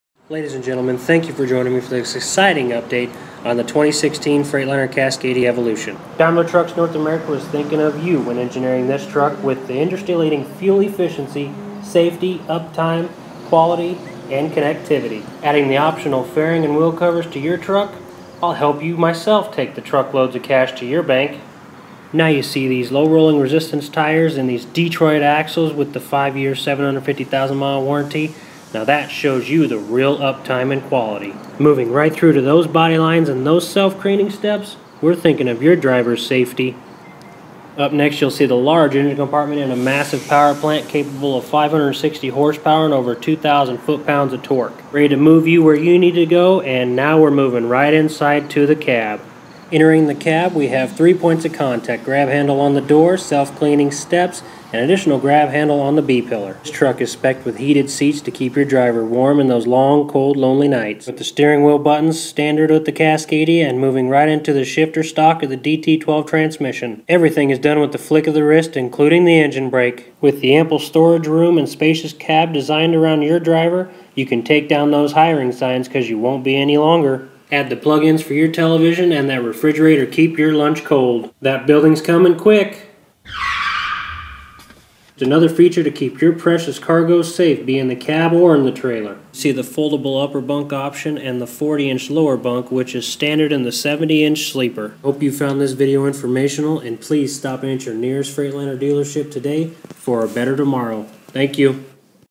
Freightliner Cascadia Evolution
Enjoy this informative walk around video on the Freightliner Cascadia Evolution, by Rocky Wray, Truck Center Companies of Lincoln, Nebraska.